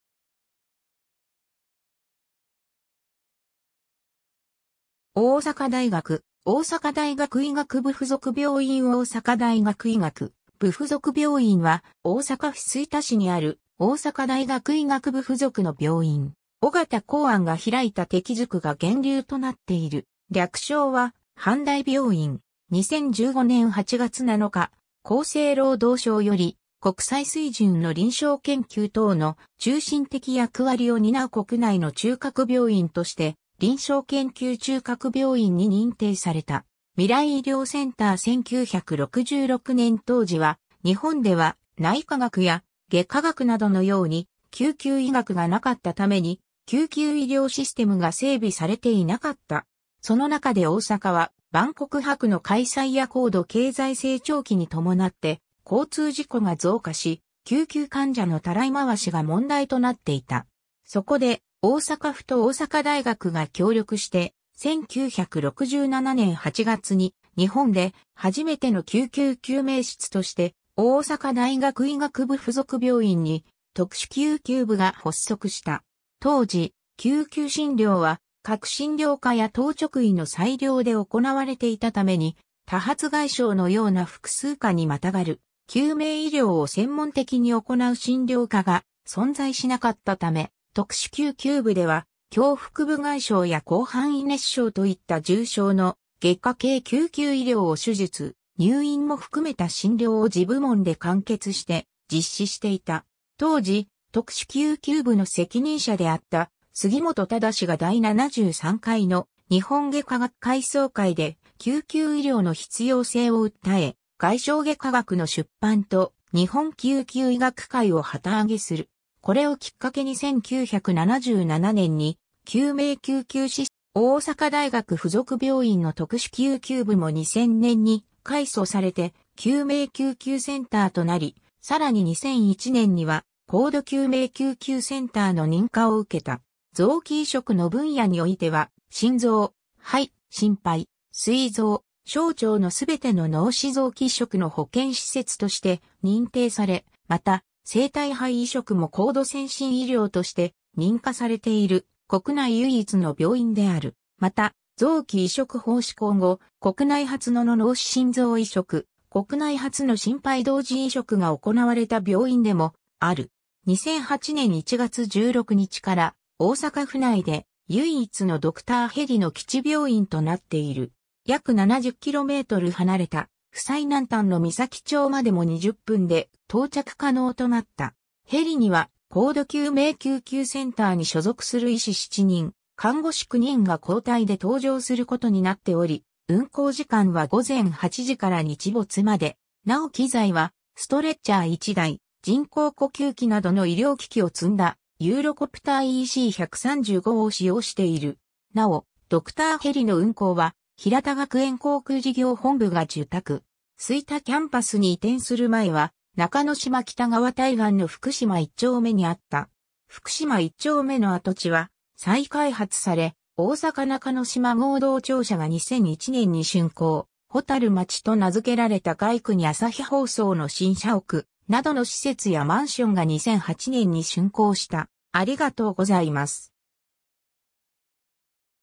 大阪大学医学部附属病院
日本の医療機関_(国立大学法人)
日本の医療機関_(医育機関)
大阪大学 」 大阪大学医学部附属病院 大阪大学医学部附属病院（おおさかだいがくいがくぶふぞくびょういん）は、大阪府吹田市にある大阪大学医学部附属の病院。
緒方洪庵が開いた適塾が源流となっている。
略称は阪大病院（はんだいびょういん）。
 2015年（平成27年）8月7日、厚生労働省より、国際水準の臨床研究等の中心的役割を担う国内の中核病院として、「臨床研究中核病院」に認定された。
 未来医療センター 1966年当時は、日本では内科学や外科学などのように救急医学がなかったために、救急医療システムが整備されていなかった。
その中で大阪は万国博の開催や高度経済成長期に伴って交通事故が増加し救急患者のたらい回しが問題となっていた。
そこで大阪府と大阪大学が協力して、1967年（昭和42年）8月に日本で初めての救急救命室として大阪大学医学部附属病院に「特殊救急部（災害外科）」が発足した。
当時、救急診療は各診療科や当直医の裁量で行われていたために、多発外傷のような複数科にまたがる救命医療を専門的に行う診療科が存在しなかったため、特殊救急部では胸腹部外傷や広範囲熱傷といった重症の外科系救急医療を手術・入院も含めた診療を自部門で完結して実施していた。
 当時、特殊救急部の責任者であった杉本侃が第73回の日本外科学会総会で救急医療の必要性を訴え、外傷外科学の出版と日本救急医学会を旗揚げする。
これをきっかけに1977年に救命救急システムが法制化されて、特殊救急部をモデルとして全国に救命救急センターが設置された。
大阪大学付属病院の特殊救急部も2000年に改組されて救命救急センターとなり、さらに2001年には高度救命救急センターの認可を受けた。
 臓器移植の分野においては、心臓、肺、心肺、膵臓、小腸の全ての脳死臓器移植の保険施設として認定され、また生体肺移植も高度先進医療として認可されている、国内唯一の病院である。
 また、臓器移植法施行後、国内初のの脳死心臓移植（1999年）、国内初の心肺同時移植（2009年）が行われた病院でもある。
 2008年1月16日から、大阪府内で唯一のドクターヘリの基地病院となっている。
約70km離れた府最南端の岬町までも20分で到着可能となった。
ヘリには高度救命救急センターに所属する医師7人、看護師9人が交代で搭乗することになっており、運航時間は午前8時から日没まで。
なお機材は、ストレッチャー1台、人工呼吸器などの医療機器を積んだユーロコプターEC135を使用している。
なお、ドクターヘリの運航はヒラタ学園航空事業本部が受託。
 吹田キャンパスに移転する前は、中之島北側対岸の福島1丁目（旧町名：堂島浜通）にあった。
福島1丁目の跡地は再開発され、大阪中之島合同庁舎が2001年に竣工、「ほたるまち」と名付けられた街区に朝日放送の新社屋などの施設やマンションが2008年に竣工した。